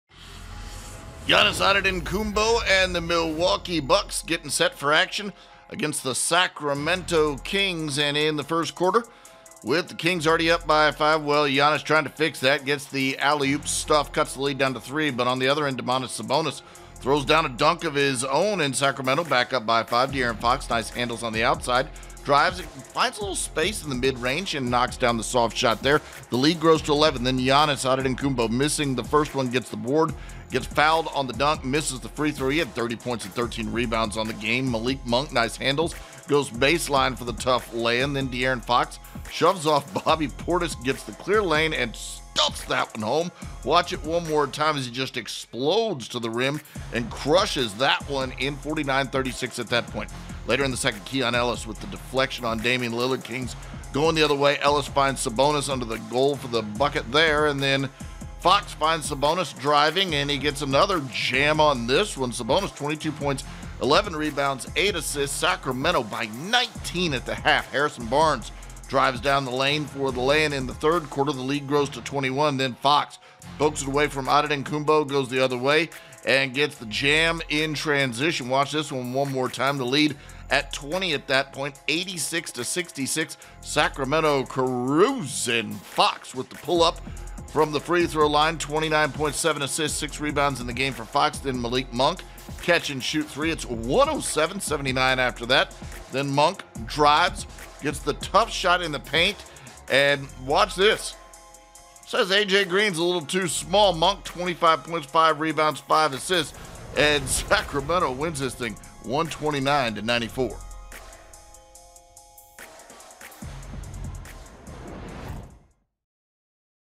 Sacramento Kings 129 x 94 Milwaukee Bucks _ March 12, 2024 Game Recap
De'Aaron Fox scores 29 points and the Kings snap a 15-game skid against the Bucks with a 129-94 win

SACRAMENTO, Calif. (AP) De'Aaron Fox scored 29 points and the Sacramento Kings beat Milwaukee 129-94 on Tuesday night for their first win over the Bucks in more than eight years.

Sacramento had not defeated Milwaukee since Feb. 1, 2016, with the 15-game losing streak being the longest active streak for any team.

“I think defensively we did a great job,” Fox said. "I think that fueled our offense. Obviously we scored 130 points, but even if we didn’t play that well offensively, if we have even below-average game, score 110, we still win this game by 15. ... I think this was one of the most complete games that we’ve played this year."

This game was never in doubt. The Kings never trailed and led by double digits since the midway point of the second quarter and built the lead to 30 points in the fourth quarter.

Domantas Sabonis had 22 points and 11 rebounds, setting a Kings single-season record with his 47th consecutive double-double. Jerry Lucas had set the mark in 1967-68.

“Night in, night out, he's come up big for us,” Fox said. “I think people are immune to it. No one outside of basically us talks about it. But that’s obviously a hard thing to do and there's not many people have done it. He comes ready to play every night.”

Malik Monk added 25 points off the bench.

The Kings have won three of four and trail Phoenix by just a half-game in the race for sixth place in the Western Conference.

Giannis Antetokounmpo had 30 points and 13 rebounds for the Bucks, who went 1-3 on a four-game California swing. Milwaukee shot 36.6% for the game and missed 32 of 39 from 3-point range.

“We deserved it tonight,” coach Doc Rivers said. “It's my fault. I didn’t get them prepared the way I should mentally. At shootaround, guys were talking about planes leaving. As a staff, we talked about it after shootaround that if our focus is not better than this morning, it’s going to be a long day. And it was a long day. So that’s on us.”

The one highlight for Milwaukee was a putback dunk in the closing minutes by rookie Andre Jackson Jr., whose head got above the rim on the play.

The Kings started fast offensively by making eight 3-pointers in the first quarter and building on the lead from there. Sacramento shot 66% in the first half and opened a 20-point lead late in the second quarter on a dunk by Sabonis.

The Kings led 75-56 at the half and were in control the rest of the way.

Both teams were short-handed, with Keegan Murray missing the game for Sacramento after spraining his left ankle on Sunday in Houston and Khris Middleton missing his 15th straight game for Milwaukee with a sprained left ankle.

Rivers said that Middleton has resumed practicing and is getting closer to making a return.

MarJon Beauchamp was also out for Milwaukee with back spasms and Sacramento's Trey Lyles left in the second quarter with a left knee injury and didn't return.

UP NEXT

Bucks: Host Philadelphia on Thursday night.

Kings: Host Los Angeles Lakers on Wednesday night.